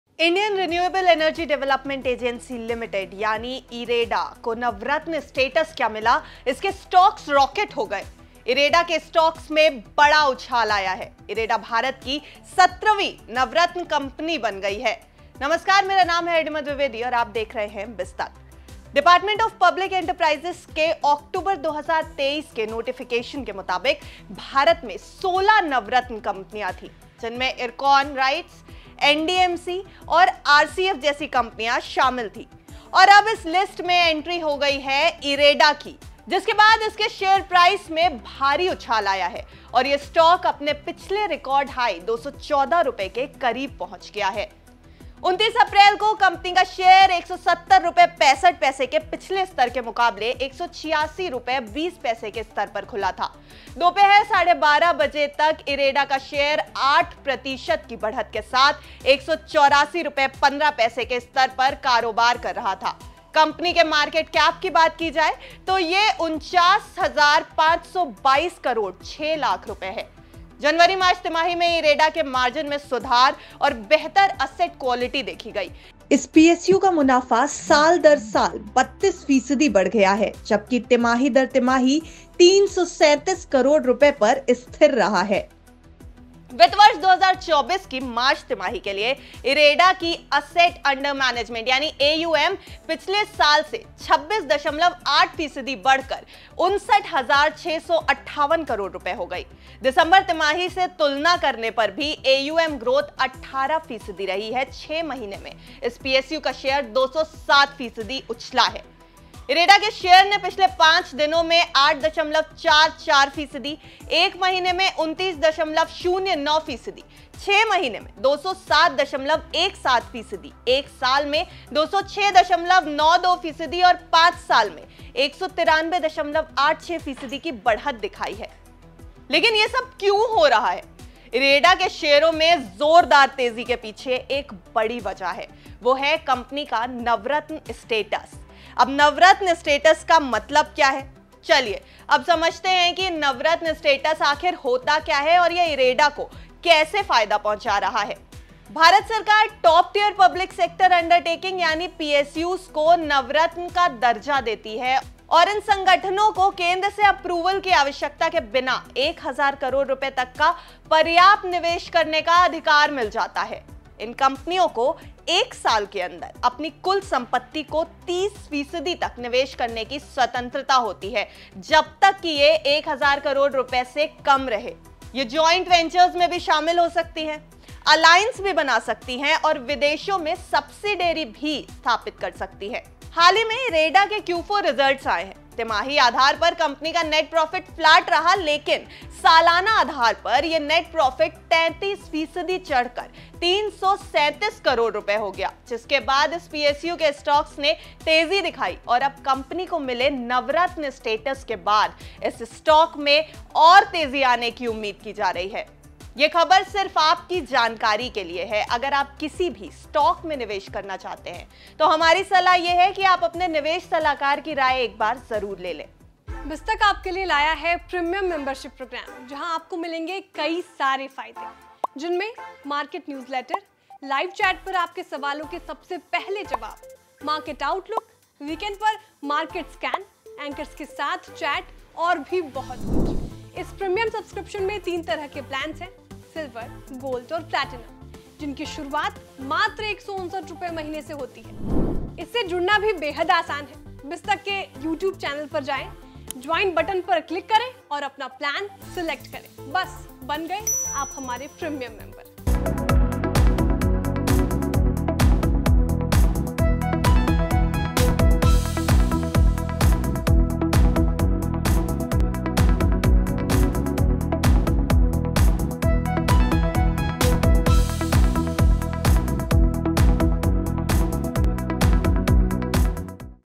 IREDA में ये क्या हो गया, अब भागेंगे Shares !| IREDA Stocks | BIZ Tak
IREDA shares surged around 13 percent in early trade on April 29 after the company achieved the 'Navratna' status. the Department of Public Enterprises (DPE) in its letter dated April 26 has granted the ‘Navratna status’ to Indian Renewable Energy Development Agency...
BizTak Premium ज्वाइन करने के लिए क्लिक करें .